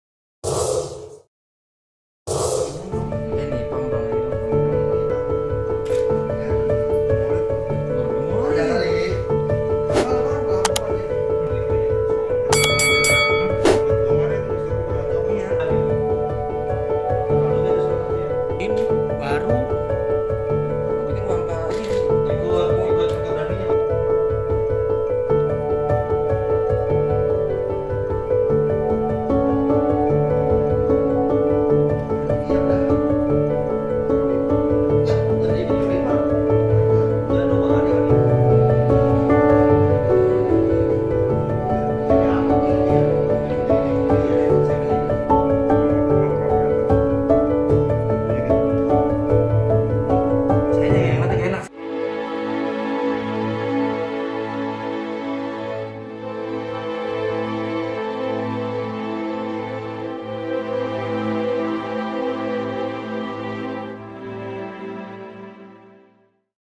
Pembuatan Miniatur Robot dari Limbah Korek Api Bekas
Miniatur robot tersebut dijual secara daring yang sudah merambah pasar mancanegara, di antaranya Jepang dan Brunei Darussalam dengan harga Rp. 50 ribu hingga Rp 2,5 juta tergantung dari tingkat kerumitannya.